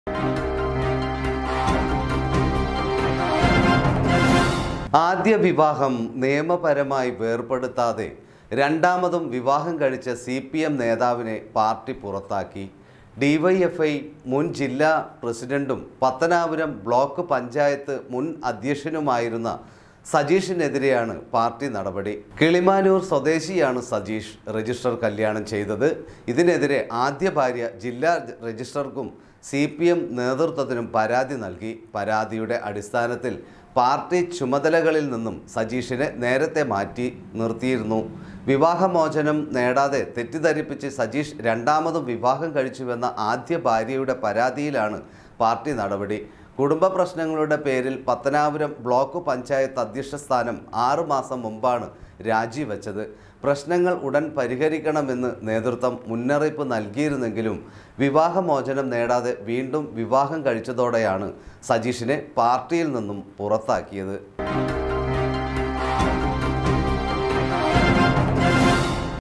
രണ്ടാമതും വിവാഹം കഴിച്ച സി.പി. എം നേതാവിനെ പാർട്ടി പുറത്താക്കി
MEDIA CITY  രണ്ടാമതും വിവാഹം കഴിച്ച സി.പി. എം നേതാവിനെ പാർട്ടി  പുറത്താക്കി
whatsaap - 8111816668